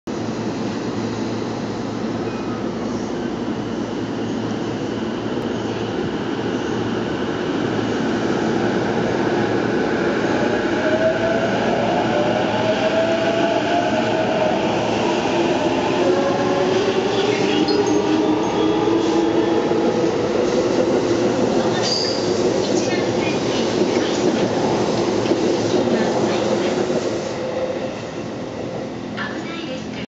Ｅ２３３系０番台(１０両) 快速 中央線
Ｅ２３３系０番台(１０両)　快速　中央線を西国分寺駅で。
T4編成の発車シーンです。